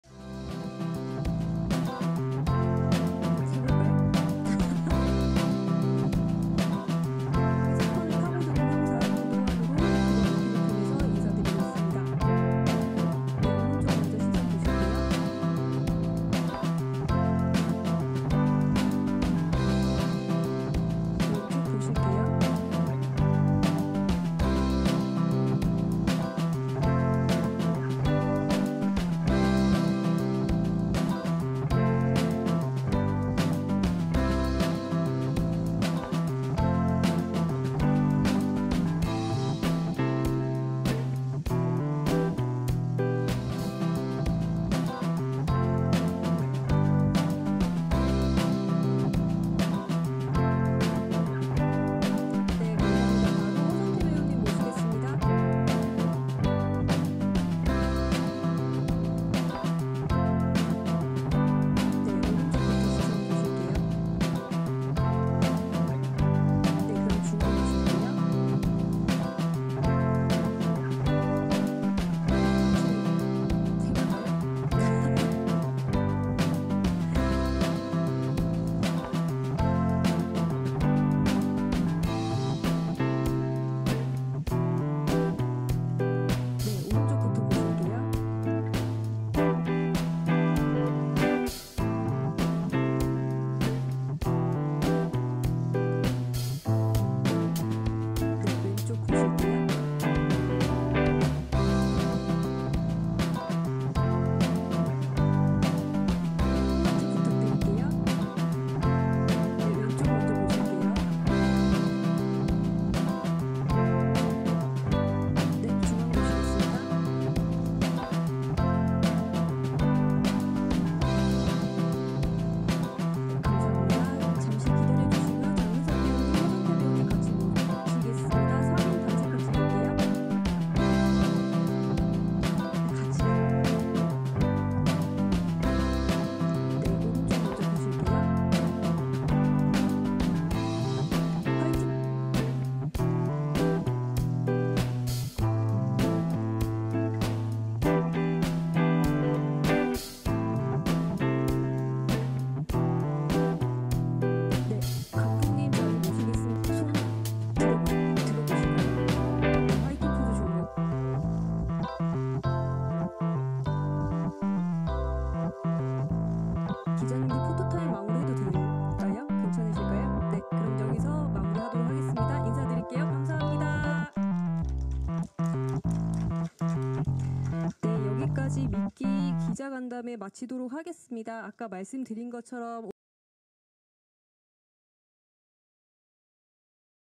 허성태 ‘마성의 하트’ 쿠팡플레이 시리즈 ‘미끼’ 언론시사회 포토타임
배우 허성태, 이엘리야, 장근석이 25일 오후 서울 메가박스 코엑스에서 열린 쿠팡플레이 시리즈 '미끼' 언론시사회에 참석해 포토타임을 가졌다.

쿠팡플레이 시리즈 '미끼'는 유사 이래 최대 사기 사건의 범인이 사망한 지 8년 후, 그가 살인 사건의 용의자로 지목되면서 이를 둘러싼 비밀을 추적하는 범죄 스릴러로 27일 첫 공개된다.